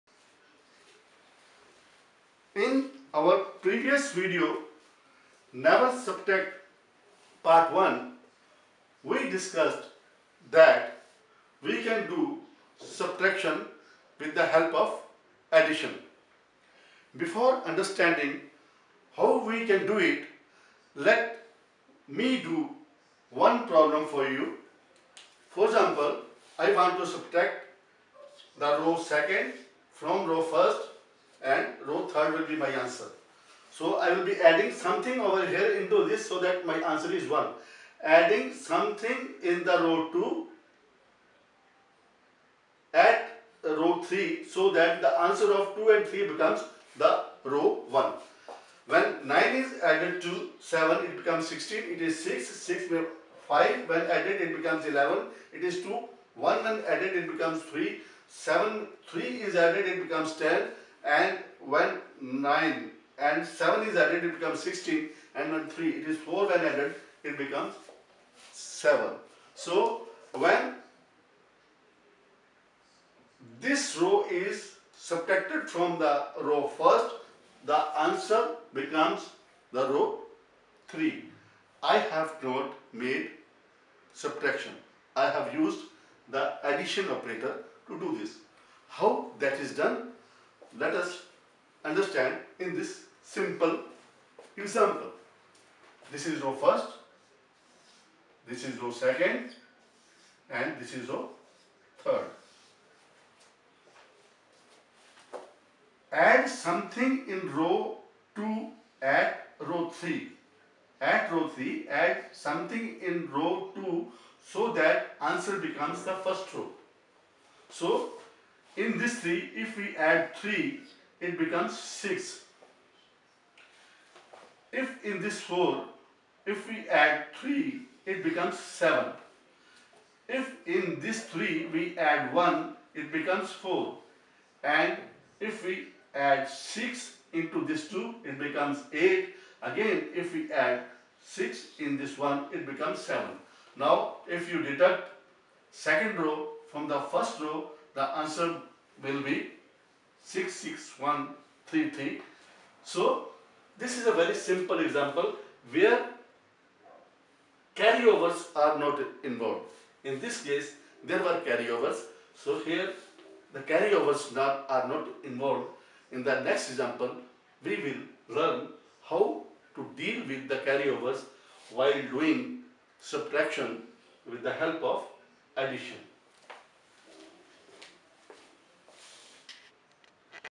Never Subtract Part 2 - A new way of doing subtraction by using '+' operator
A better and more accurate way of doing subtraction